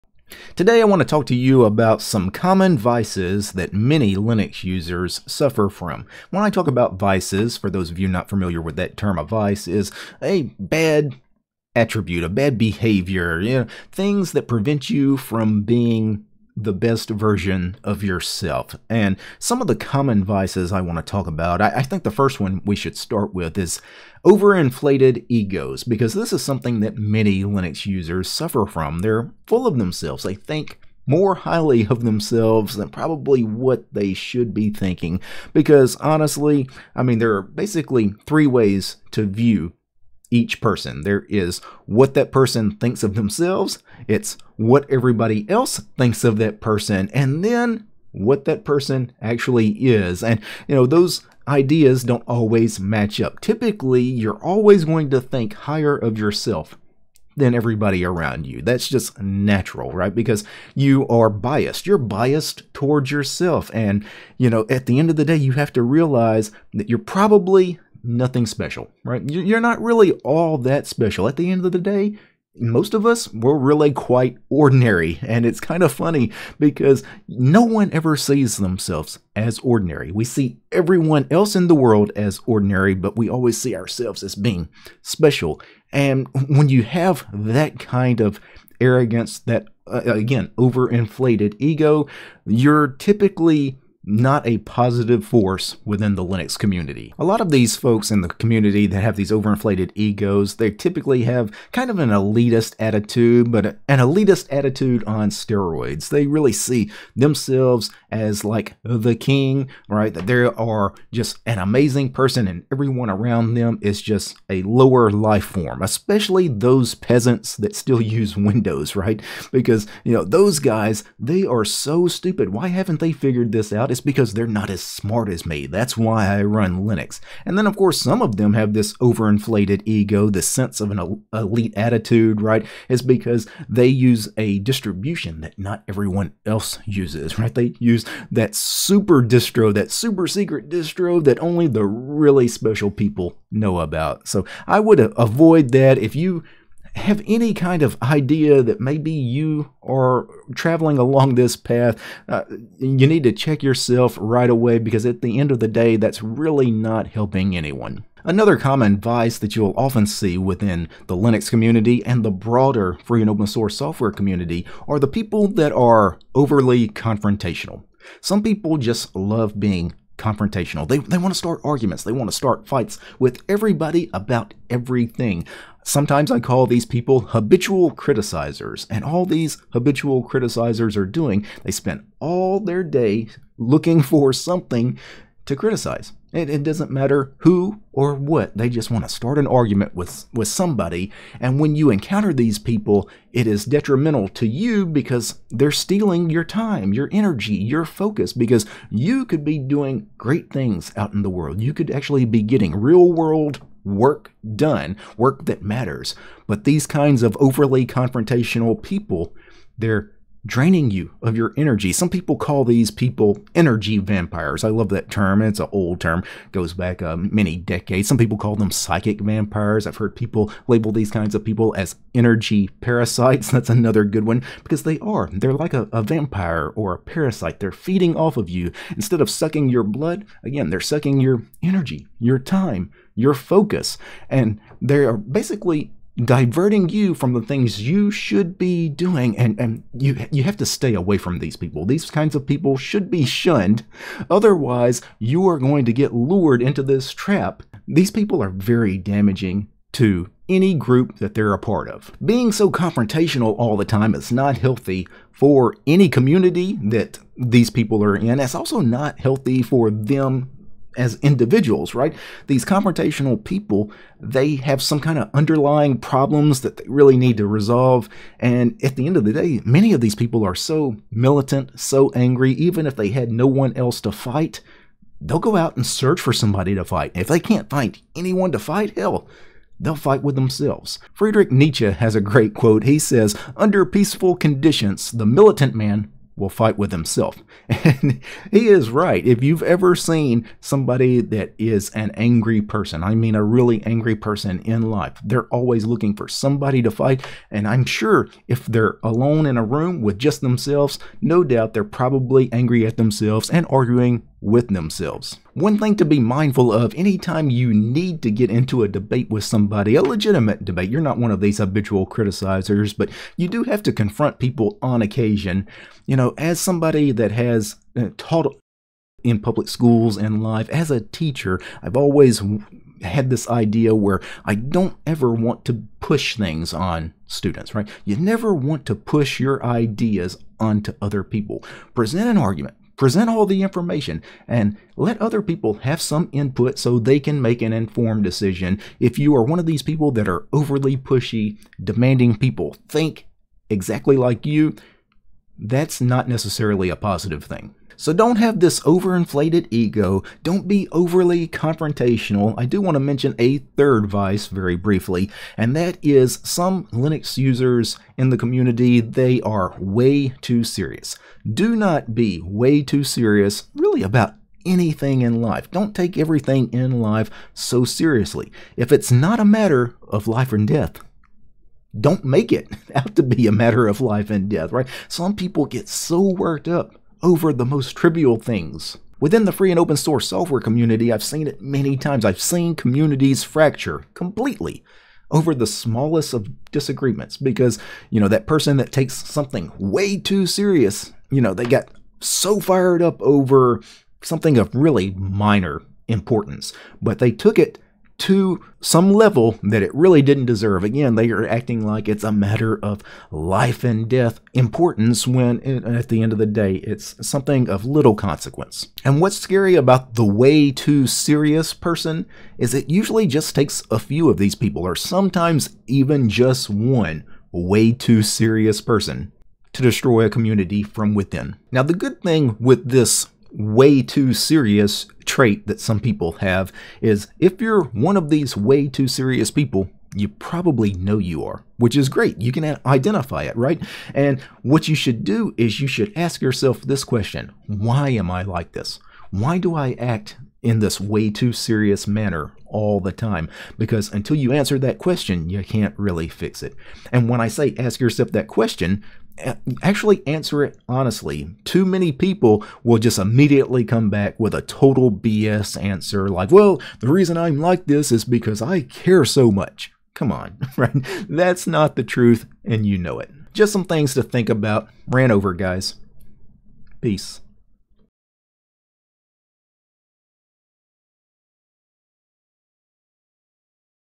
Three Common Vices Found In The Linux Community
Too many within the Linux community and the broader free and open source community have these vices.

► 0:00 - Intro
► 0:18 - Over-inflated ego
► 2:38 - Being confrontational
► 5:55 - Being way-too-serious